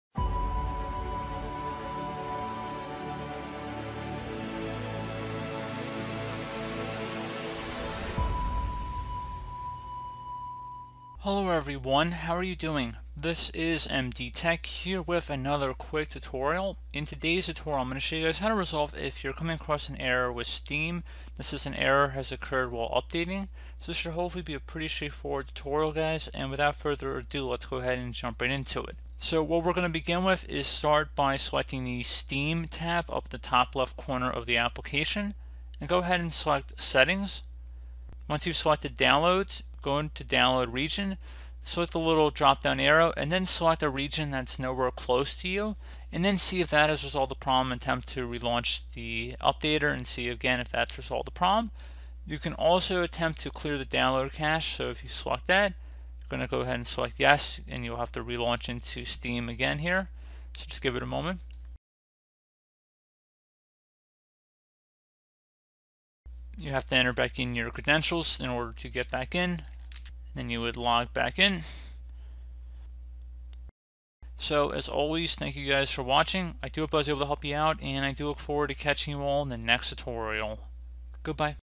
How to Fix Steam an Error Occurred While Updating FIX
Steam offers automatic updates for the installed games. Sometime, you may encounter an error while installing or updating a game and steam displays “an error occurred while updating” error”.

The full error reads An error occurred while updating [game title] or An error occurred while installing [game title]. This is a common Steam error and here are a couple of ways to troubleshoot this error on the Windows system.

This error appears anytime Steam is unable to download and save game data to the storage drive on your computer during an update or new installation. To fix the error, you need to identify why Steam is unable to save data to your storage drive and then fix that problem.